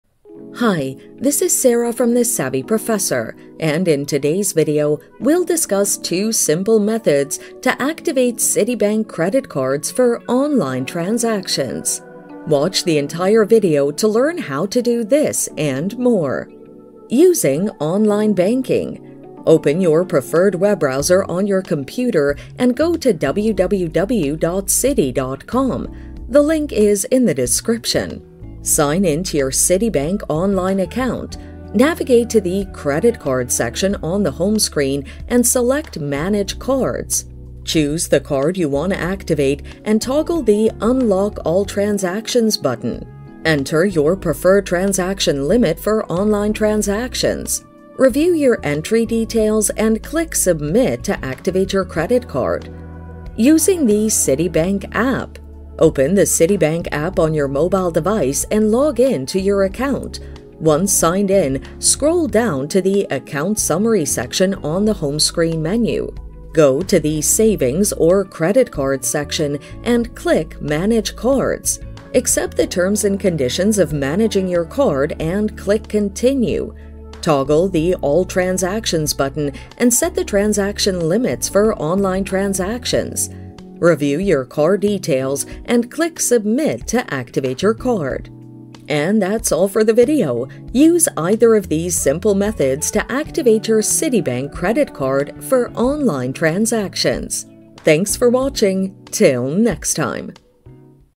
How To Activate Citibank Credit Card For Online Transactions(Enable Citibank CC Online Transactions)
How To Activate Citibank Credit Card For Online Transactions(Enable Citibank CC Online Transactions). In this video tutorial I will show you how to activate Citibank credit card for online transactions.

Using Online Banking:
• Open your preferred web browser on your computer and go (The link is in the description).
• Sign into your Citibank Online account.
• Navigate to the "Credit Card" section on the home screen and select "Manage Cards."
• Choose the card you want to activate and toggle the “Unlock all transactions” button.
• Enter your preferred transaction limit for online transactions.
• Review your entry details and click “Submit” to activate your Credit Card.

Using the Citibank app:
• Open the Citibank app on your mobile device and log into your account.
• Once signed in, scroll down to the “Account Summary” section on the home screen menu.
• Go to the “Savings/Credit Card” section and click “Manage Cards”.
• Toggle the “All transactions” button and set the transaction limits for online transactions.
• Review your details and click “Submit” to activate your card.